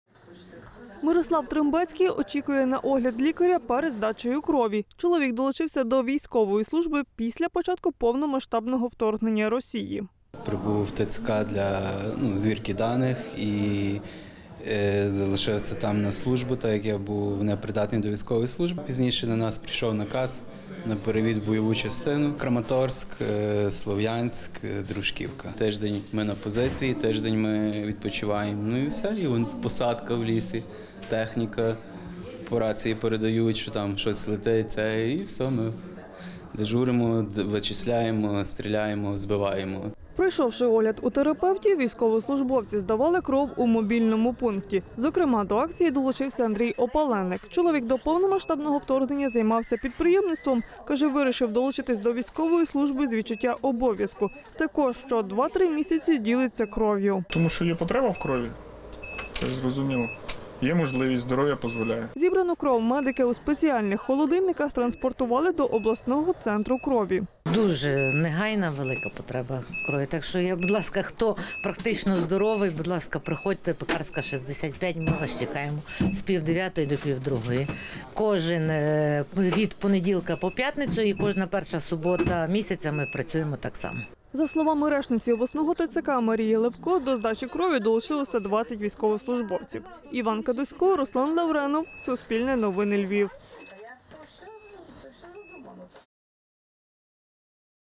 Як військовослужбовці з ТЦК здають кров у донорському центрі
В обласному центрі служби крові у Львові бракує донорської крові. Сьогодні до поповнення запасів долучились військовослужбовці Сихівського районного Територіального центру комплектування та соцпідтримки. Військовослужбовець Андрій Опаленик розповів, що здає кров кожних 2-3 місяців, адже відчуває обов'язок перед побратимами. Більше дивіться у сюжеті.